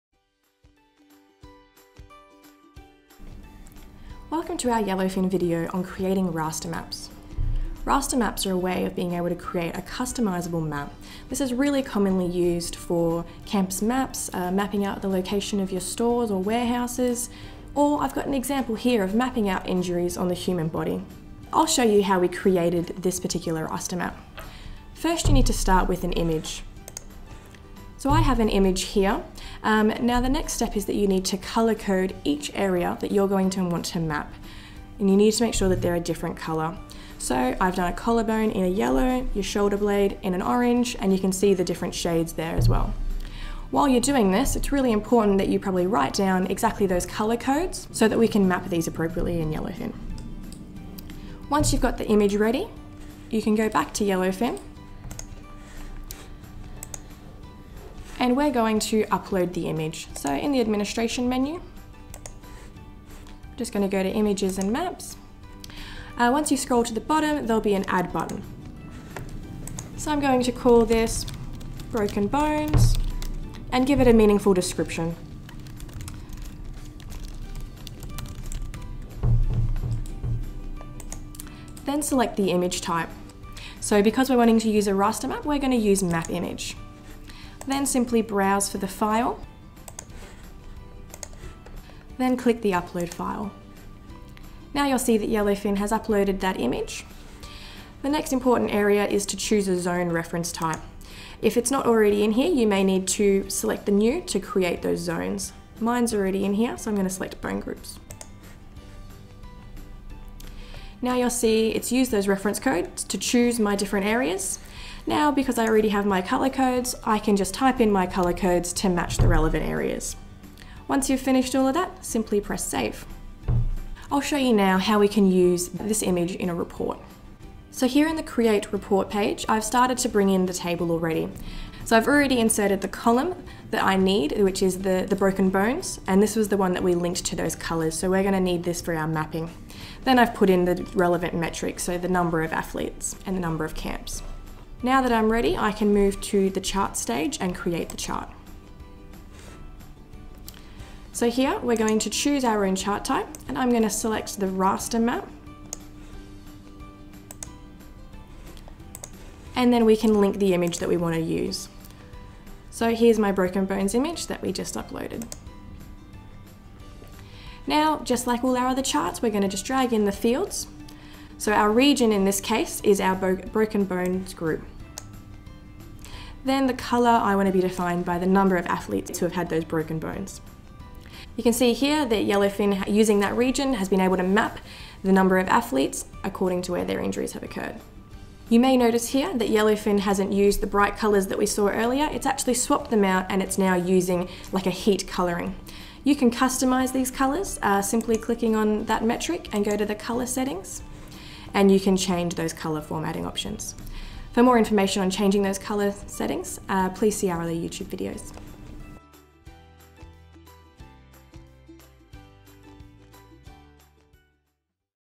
How to use Images for Floorplans and Thematic Maps - Yellowfin
This is an official Yellowfin training video to demonstrate how to create custom maps for floorplans, etc.